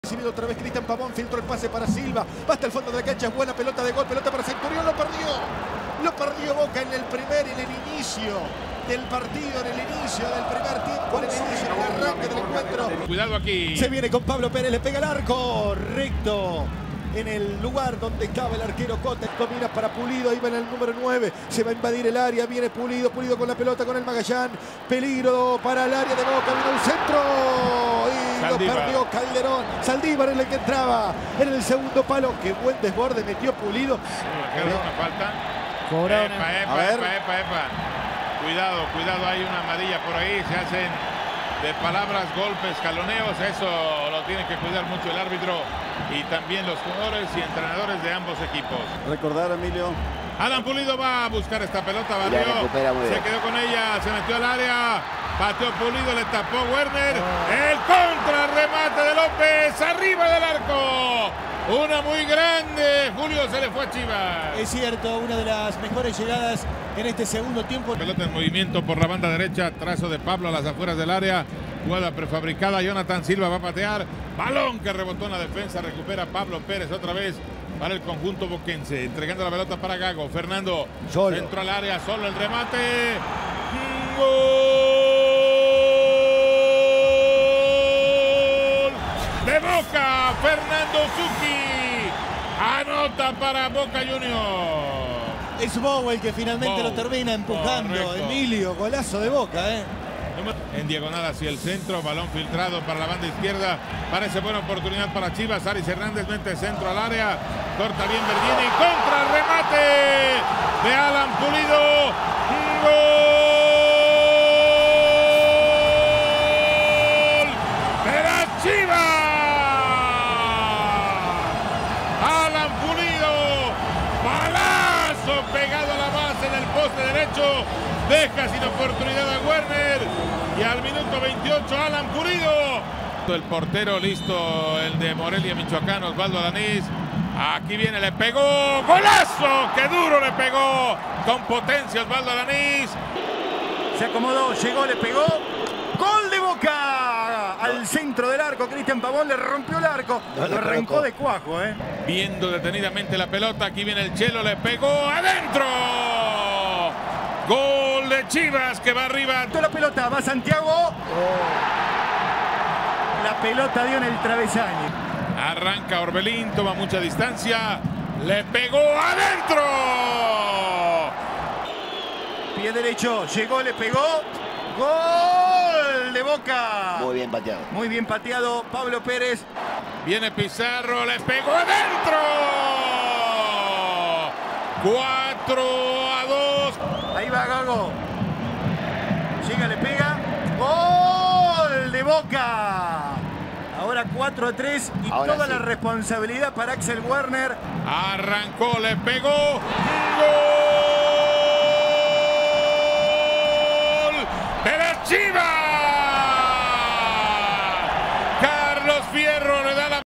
Chivas 1 (5) - (3) 1 Boca, el resumen| FitBood
el equipo de Guillermo Barros Schelotto ante el equipo mexicano dirigido por Matías Almeyda en la definición por penales de este amistoso internacional que enfrentaba a dos grandes del continente. GENTILEZA CHIVAS TV.